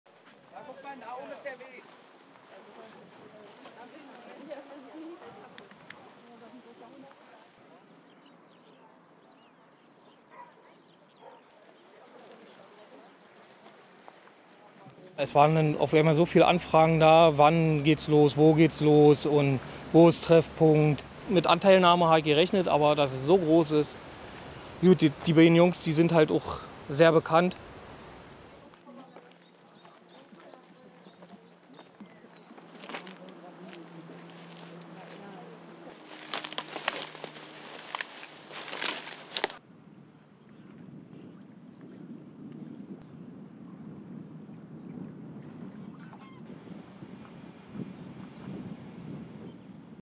Zwei junge Männer nach Herrentagsparty VERMISST
Sie sind seit einer Party am Herrentag verschwunden. Jetzt suchen mehr als 150 Freiwillige nach den zwei 22-Jährigen aus Zehdenick. Die Polizei setzt auch Spezialhunde ein. Am Ufer des Großen
Wentowsees wurden Kleidungsstücke der Vermissten gefunden.